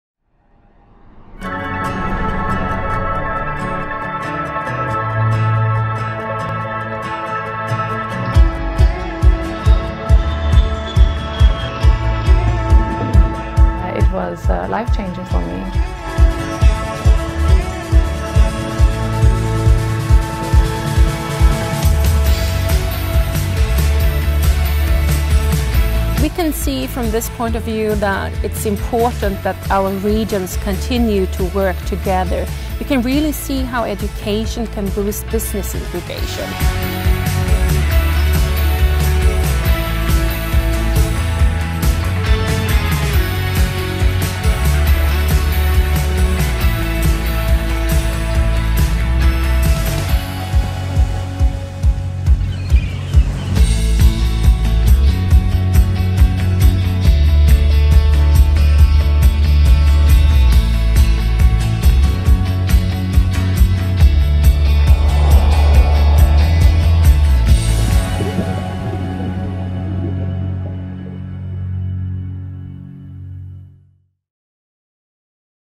South Baltic in Moving Images - trailer - South Baltic Programme
South Baltic in Moving Images is supporting the dissemination and capitalisation activities of the Programme, notably by showcasing the added-value of Southern Baltic Sea cooperation through selected project results from the programming period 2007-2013 and their impact on the life of our fellow-citizens, the economy and the environment.